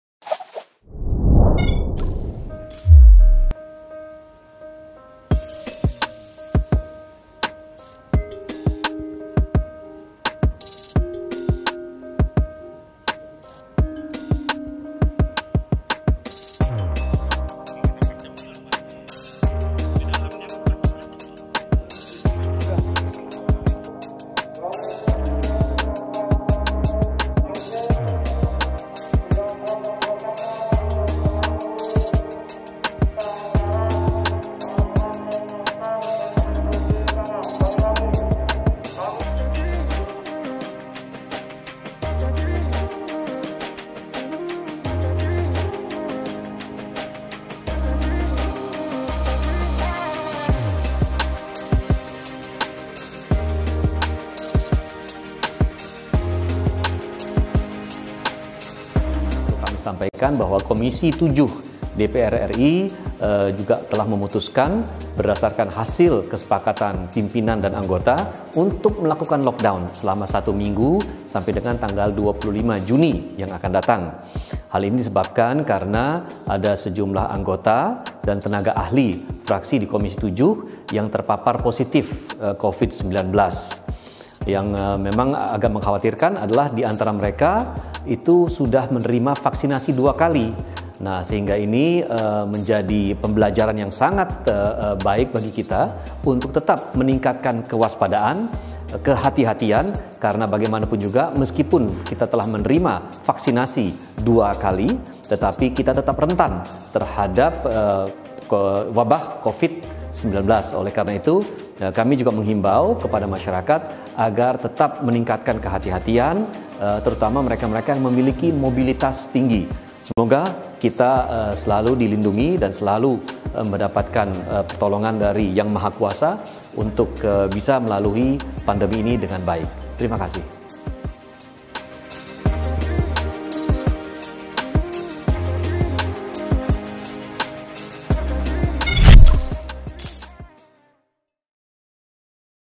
Anggota dan Tenaga Ahli Positif Corona, Komisi VII DPR Dilockdown
Giliran Komisi VII DPR RI dilockdown setelah ditemukan sejumlah anggota dan tenaga ahli yang terkonfirmasi positif Covid-19. Setelah sebelumnya komisi I dan komisi VIII DPR RI telah lebih dahulu menerapkan lockdown di ruangannya.